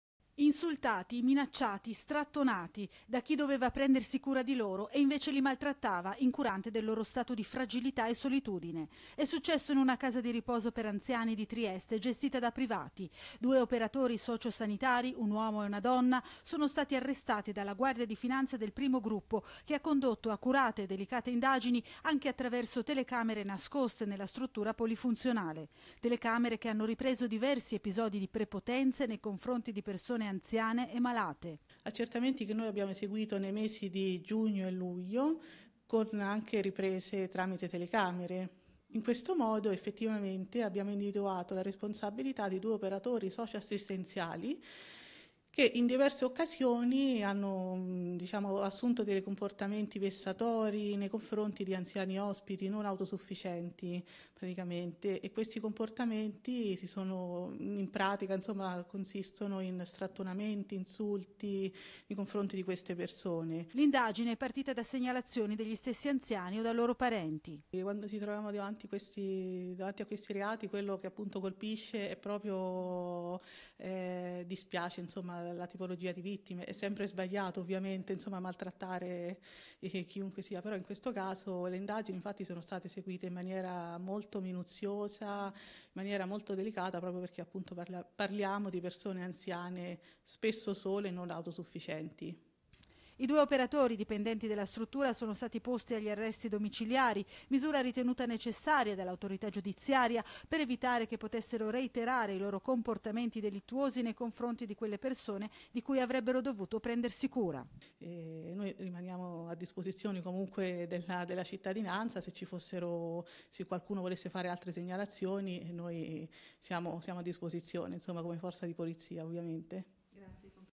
08/08/2018 - INSULTAVANO E MALTRATTAVANO ANZIANI IN CASA DI RIPOSO: ARRESTATI DUE OPERATORI
IL NOTIZIARIO (mercoledì 8 agosto 2018) - Insultati, minacciati, strattonati. Da chi doveva prendersi cura di loro  e invece li maltrattava, incurante del loro stato di fragilità e solitudine.  E' successo in una casa di riposo per anziani di Trieste gestita da privati. Due operatori socio sanitari, un uomo e una donna, sono stati arrestati dalla Guardia di Finanza del primo gruppo, che ha condotto accurate e delicate indagini, anche attraverso telecamere nascoste nella struttura polifunzionale.
Telecamere che hanno ripreso diversi episodi di prepotenze nei confronti di persone anziane e malate.